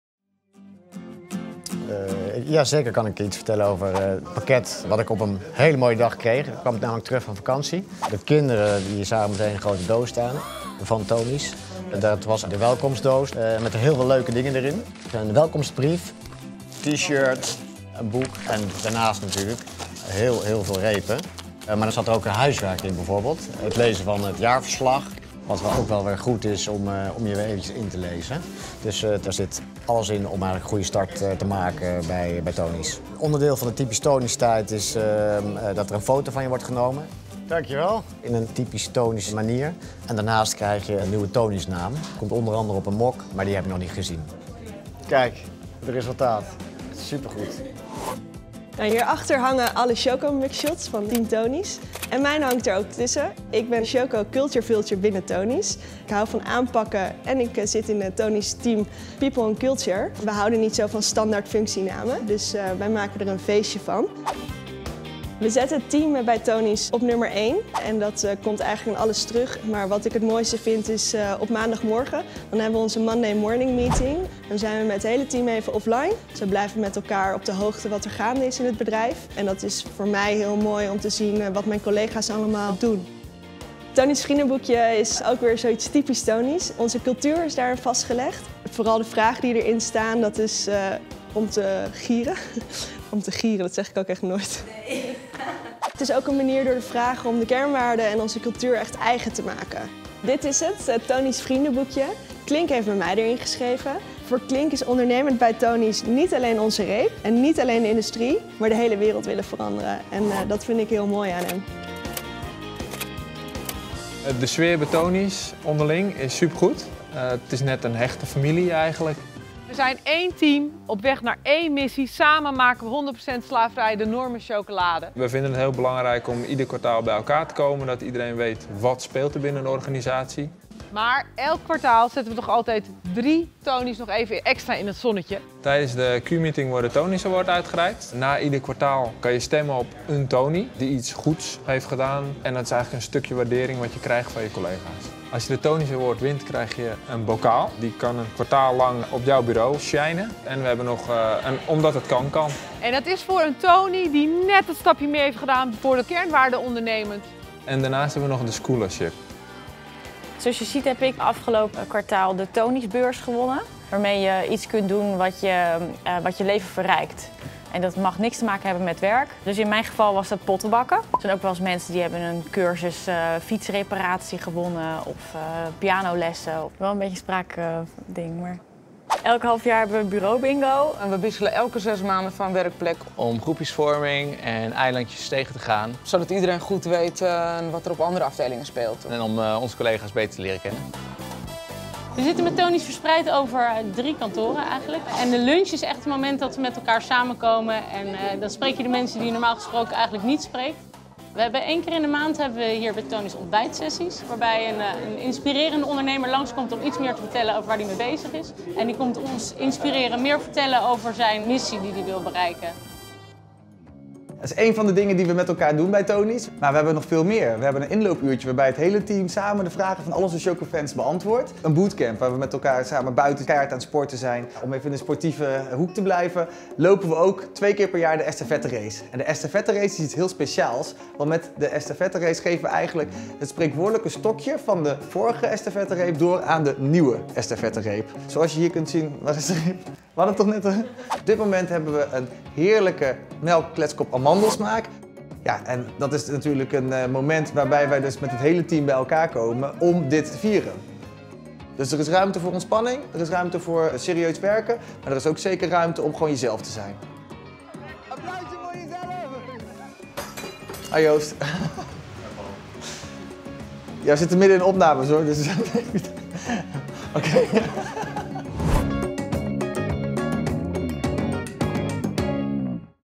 Tony's Chocolonely - a peek into our choco office
Curious how things work at Tony's? Check it out, for a peek into our (choco) homebase. // NL: Benieuwd hoe 't er bij Tony's op de werkvloer aan toegaat? Kijk maar, voor 'n kijkje in onze (choco)keuken.